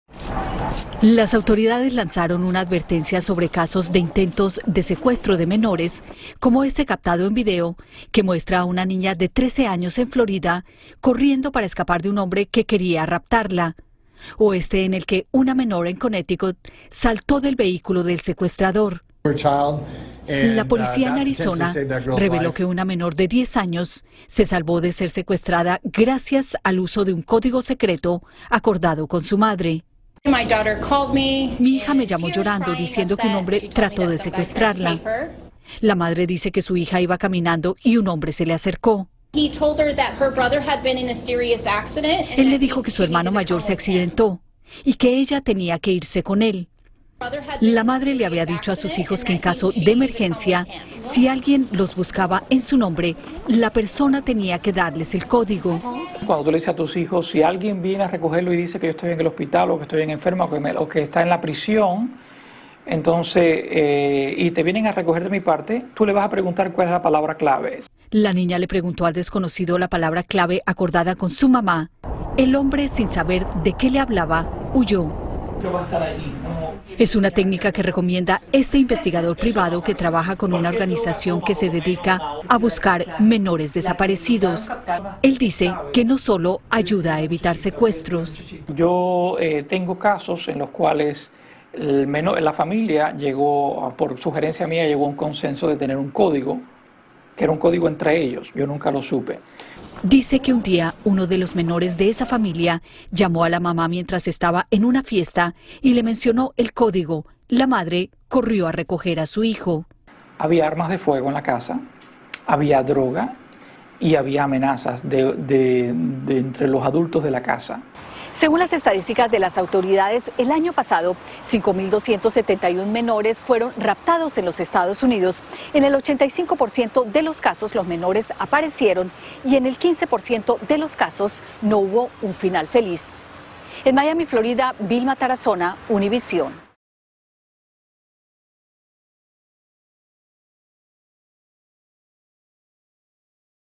Una palabra clave salvó a una niña de un secuestro en Arizona
La niña de 10 años iba caminando cuando un extraño se le acercó a decirle que su hermano sufrió un accidente y que tenía que irse con él. La madre de la pequeña le había dado indicaciones de pedir la palabra clave en caso de que algún desconocido fuera a buscarla de su parte en caso de una emergencia. El hombre sin saber de que le hablaba la niña huyó. Un investigador que se dedica a buscar a niños desaparecidos nos indicó que esta es una muy buena técnica que todo padre debe aplicar.